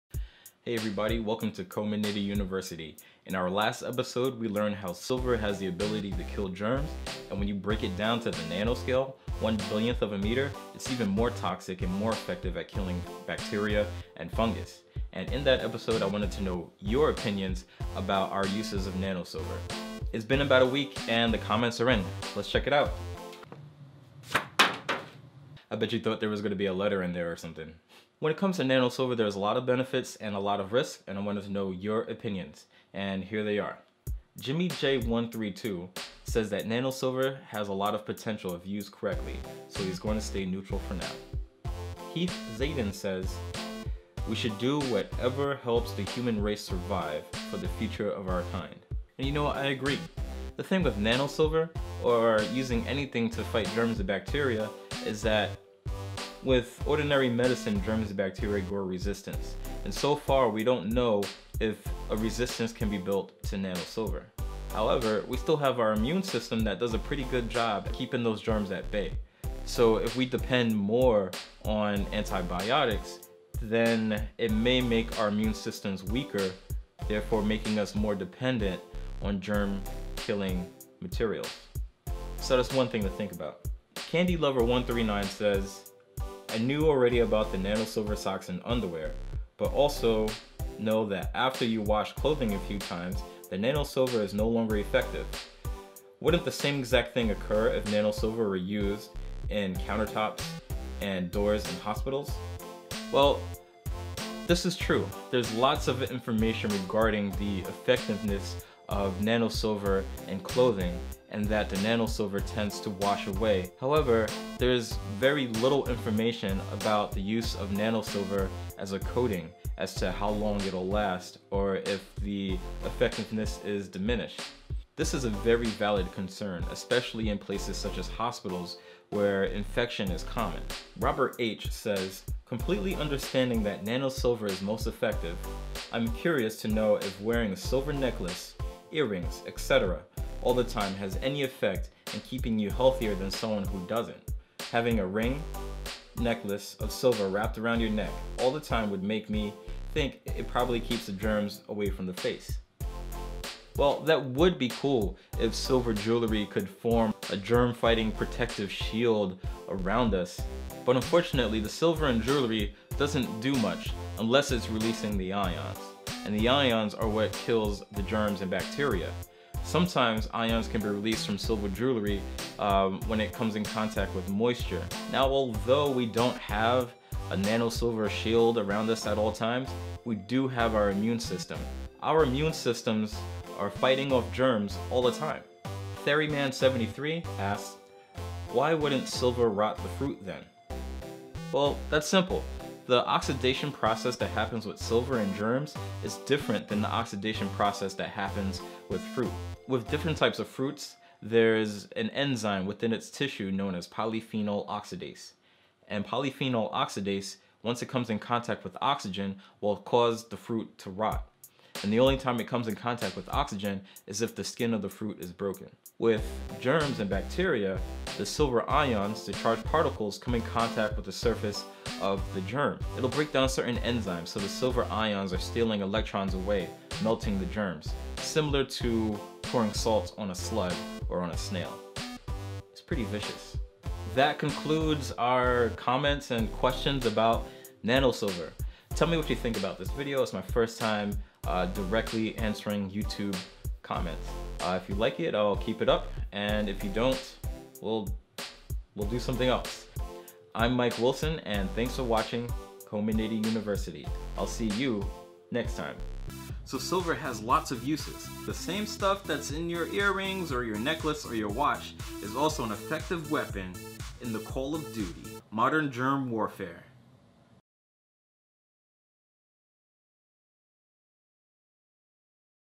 Nano-Silver Science Comments - Coma Niddy University
In our last episode we learned that silver has the ability to kill germs, fungus and bacteria. Silver's antimicrobial abilities come at a price and I wanted to know you opinions on it. I discuss and answer some of your comments!

This is my first time directly responding to your comments. Let me know how you like it!

Credits:
Starring Mike Wilson aka Coma Niddy
Camera, Music, Post Production, by Mike Wilson aka Coma Niddy